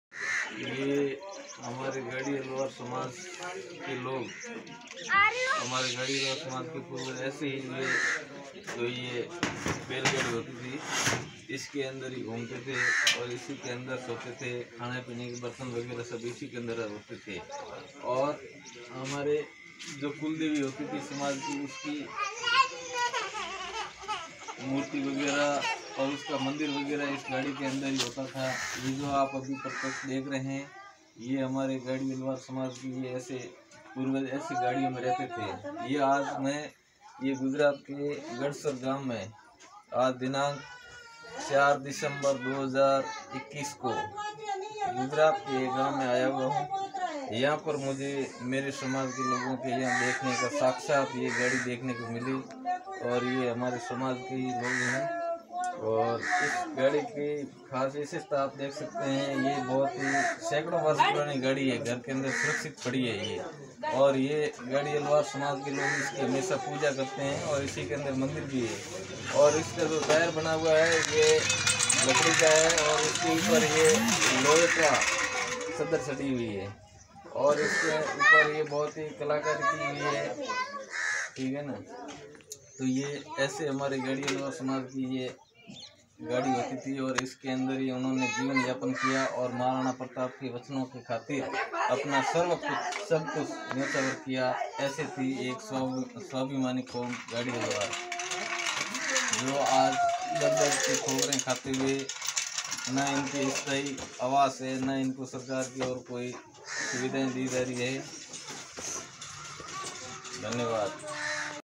#500 वर्ष पुरानी बैलगाड़ी है साथियों जिस पर गाड़ियालोहार रोजी रोटी कमाने गांव- गांव घूमते थे ।
#500 वर्ष पुरानी बैलगाड़ी है  साथियों जिस पर गाड़ियालोहार रोजी रोटी कमाने गांव- गांव घूमते थे । महाराणा प्रताप के वचनों की खातिर गांव-गांव में घूमना स्वीकार किया लेकिन मुगलों की अधीनता स्वीकार नहीं की ।